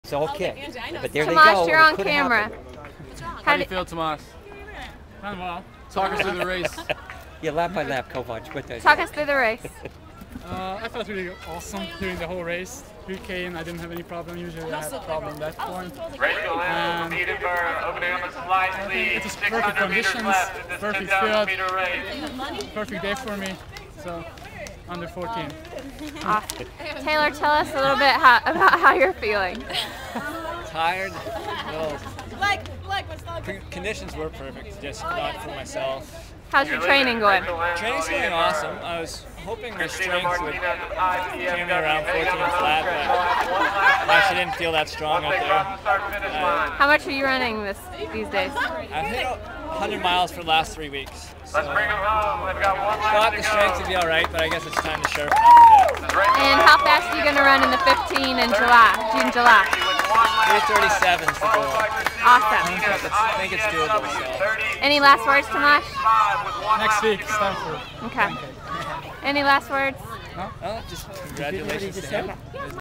Sub 14
Interview with High Point's Tamas Kovacs and High Point Alum Taylor Milne post Raleigh Relays 5k